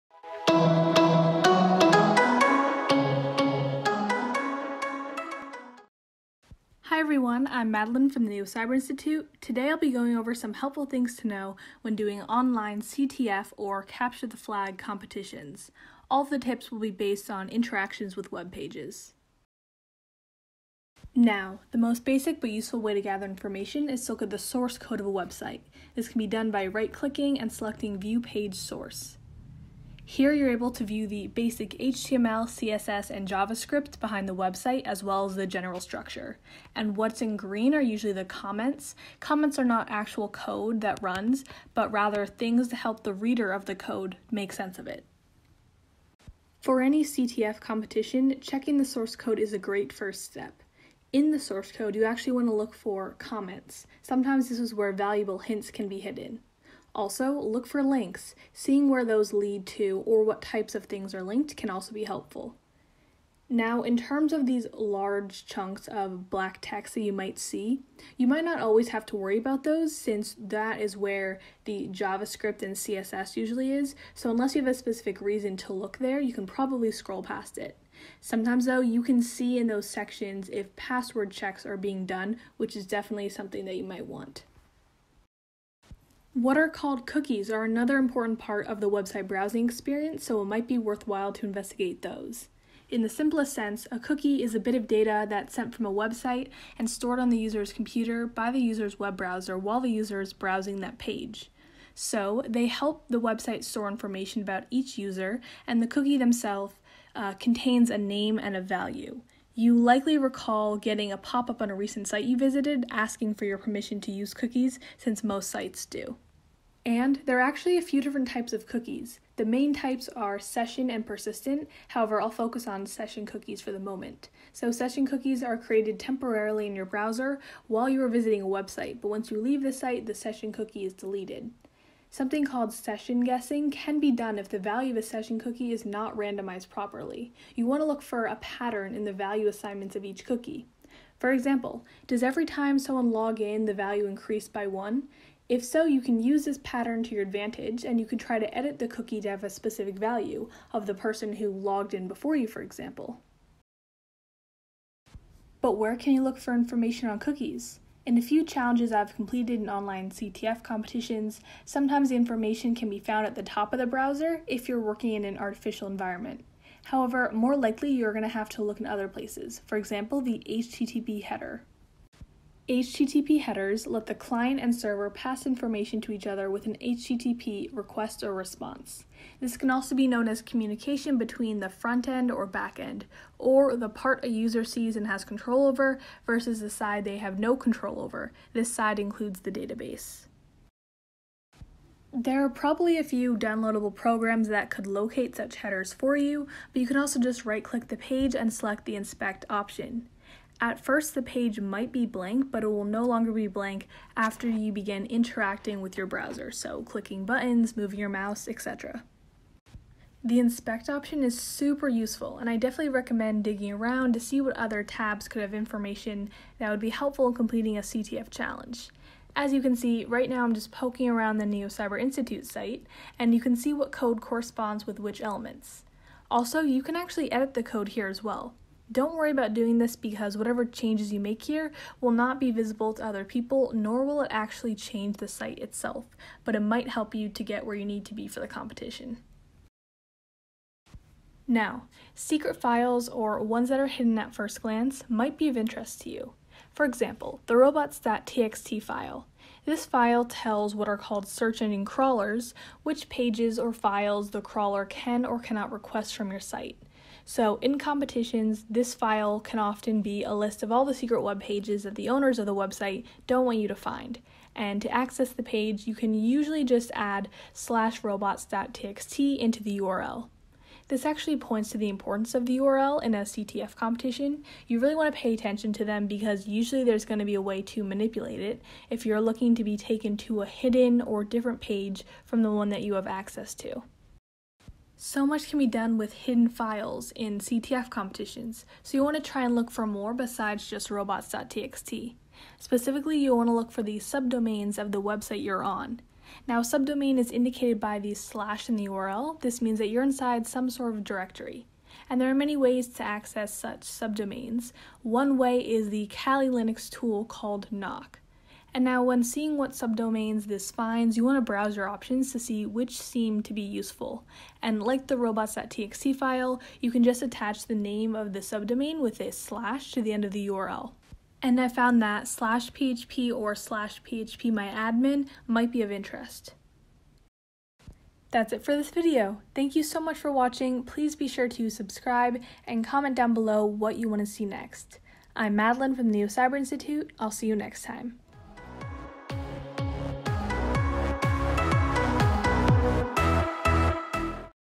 Capture the Flag: Exploring Web Pages
While this video is about intermediate topics, we also post videos about good cyber practices that everyone should have! In this video though, we look at different aspects of a website that you can explore when completing challenges in a CTF competition. These include the source code, cookies, secret files, and more.

--Timestamps--
Source code: 0:23
Cookies: 1:27 (quick note: check the "Applications" tab to see website cookies as well!)
More on the "Inspect" option: 3:34
Secret files: 4:01

Thumbnail art is from Pixabay (user krzysztof-m)